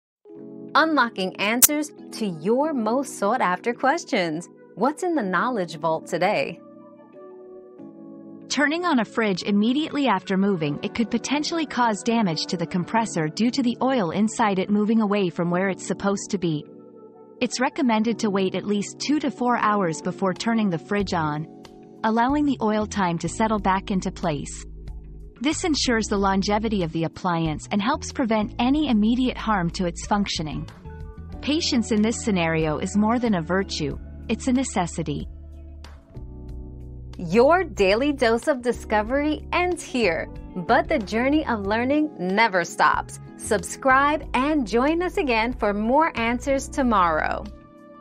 What happens if you turn on a fridge immediately after moving it?
Fridge Moving Mistake: Turn On Delay 👉 Turn On Delay 👉 Avoid damaging your fridge compressor by waiting 2-4 hours before turning it on after moving. Patience ensures longevity and proper functioning of the appliance.

Laura S. Harris (2024, April 25.) What happens if you turn on a fridge immediately after moving it?
    🌐 AskAbout.video